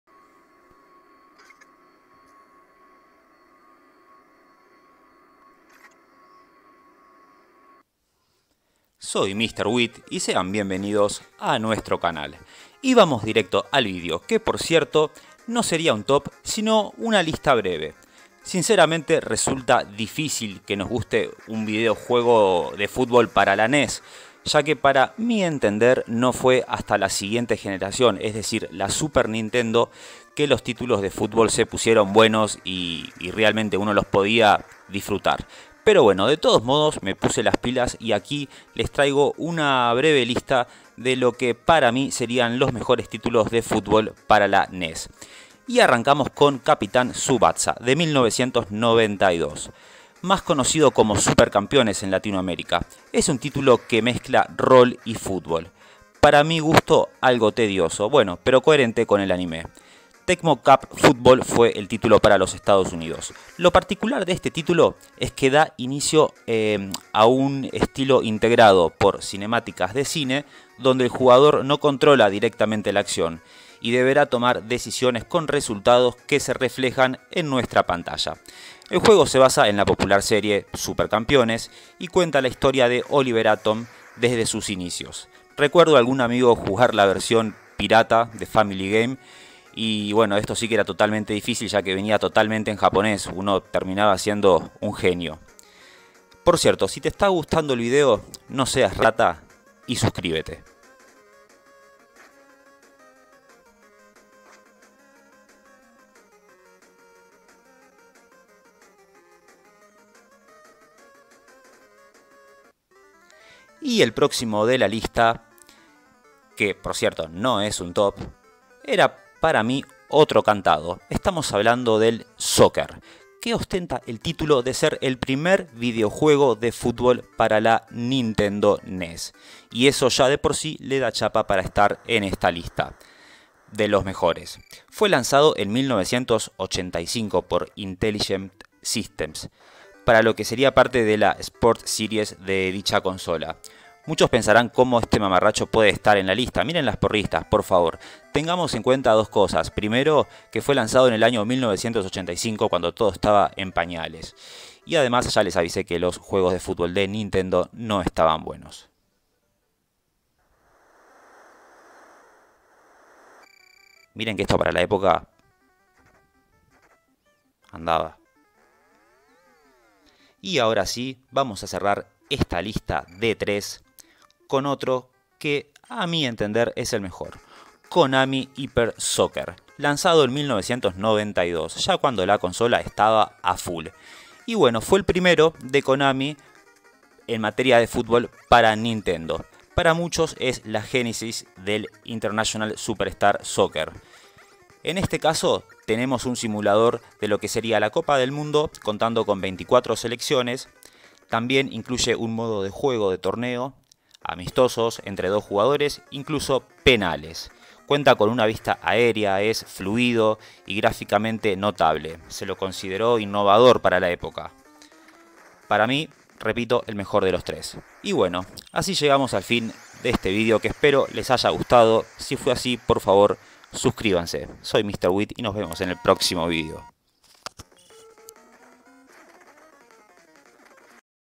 Mejores VideoJuegos De Fútbol Para NINTENDO NES Mejores Títulos De Fútbol 8 Bits
Este video no contiene material ofensivo y es apto para todo publico en este caso les traigo una lista personal con los mejores títulos de fútbol para NES , espero les guste el video y si es así por favor SUSCRIBETE
Mr Witt
Gastón Witt
Redes : Gastón Witt